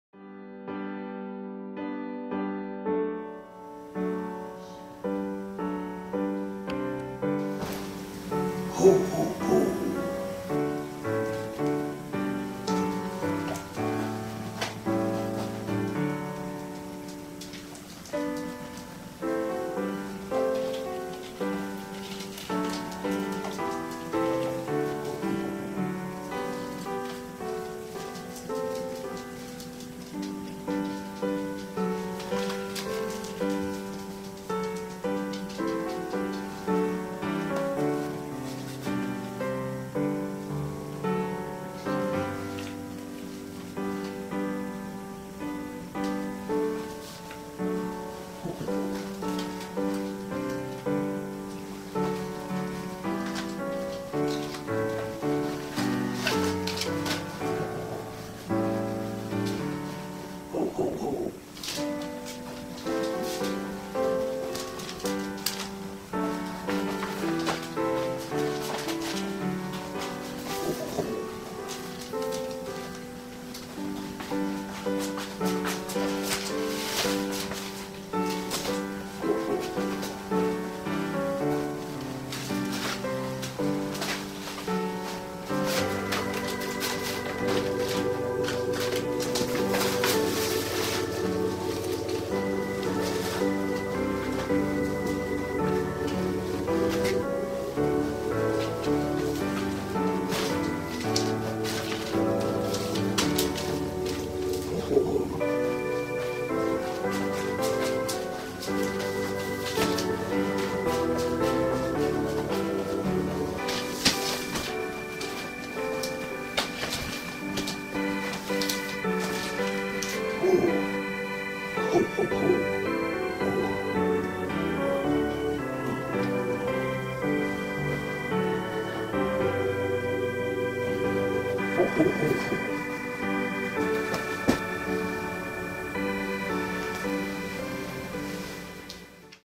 The Real Santa Claus Caught on Tape / Camera /Video 4K Ultra HD Enters Home Eats Cookies drinks Milk
Real Santa Claus Caught on Tape / Camera /Video 4K Ultra HD Enters Home Eats Cookies and drinks Milk. Crescendo Kids, MinetheJ kid gamer, and Jaden set up hidden camera and catch The Real Santa entering Home and stuffing stockings and delivering presents. Hear him Ho Ho Ho and disappear. Wow very cool!!
Kid gamer's house MinetheJ, Jaden Crescendo gets some games from Santa.